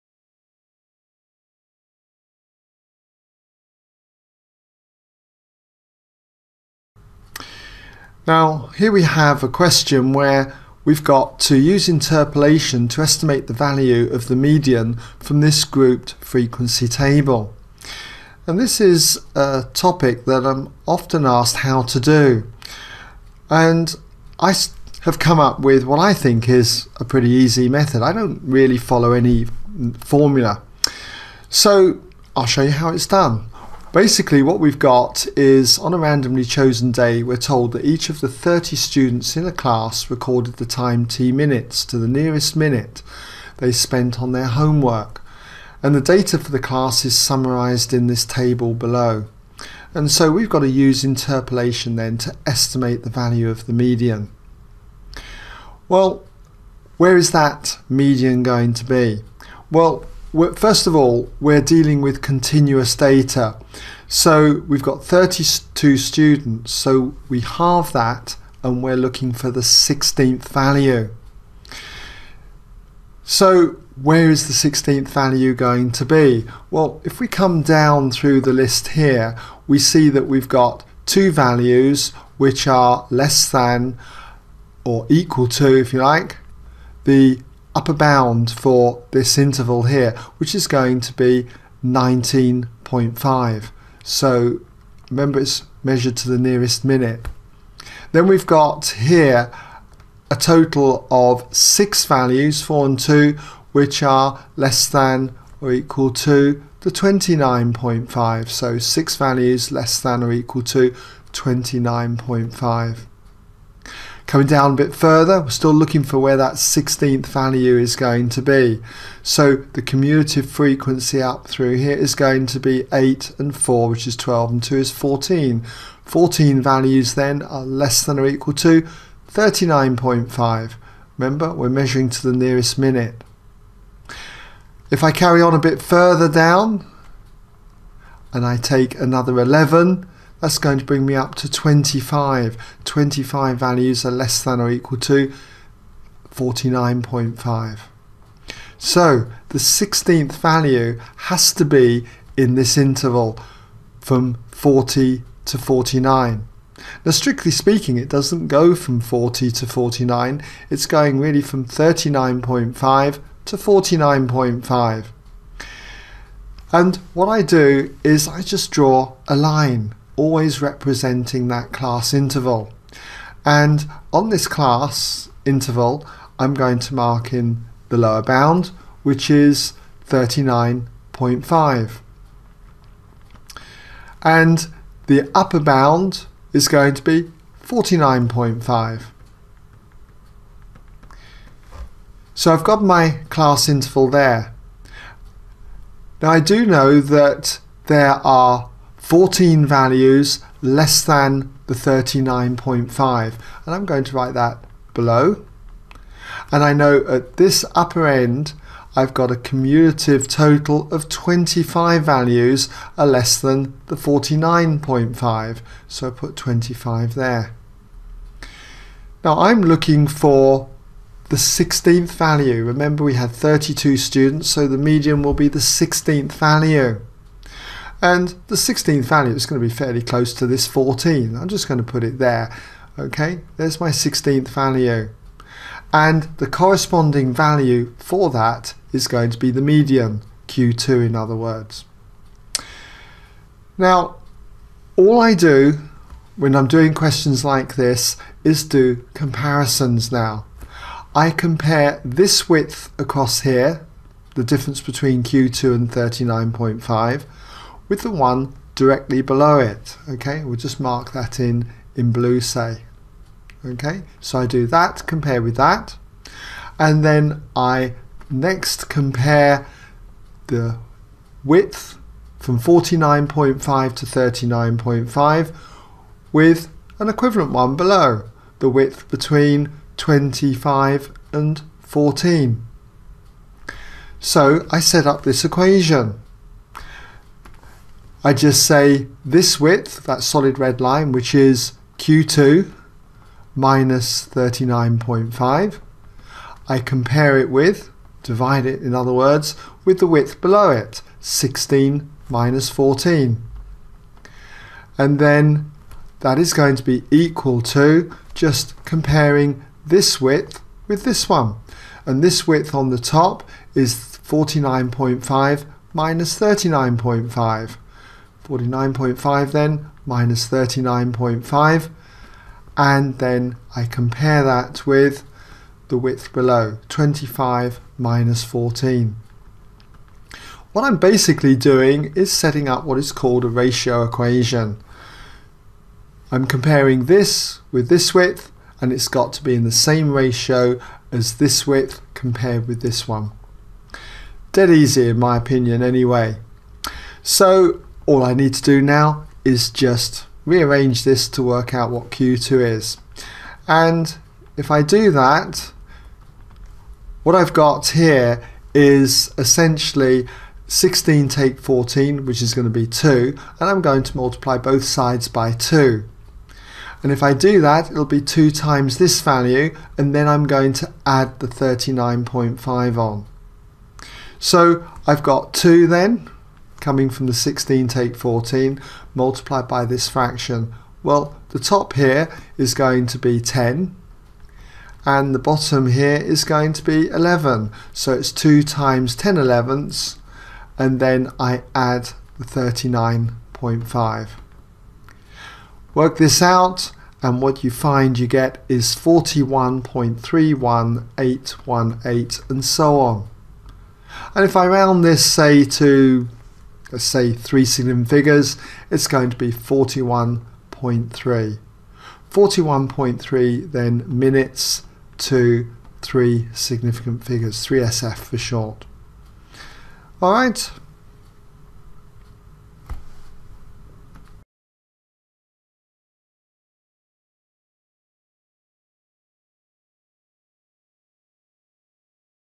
Estimating a Median by Linear Interpolation | Edexcel S1 January 2011 Q5a | ExamSolutions
This is a model answer to the above paper.